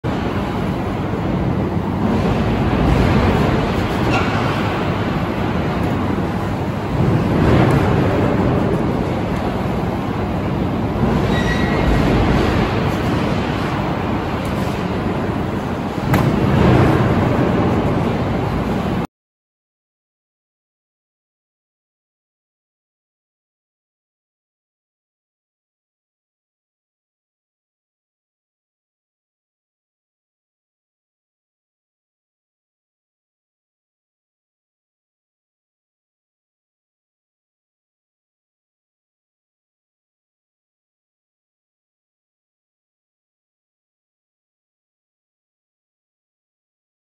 Jednokřídlé samonosné posuvné dveře / Single-leaf self-supporting sliding door
Výrobce/dodavatel: AMTEK, spol s.r.o.

MayTec – ALU profile system

Lokace: Škoda Auto a.s. - Mladá Boleslav, hala lisovny M12A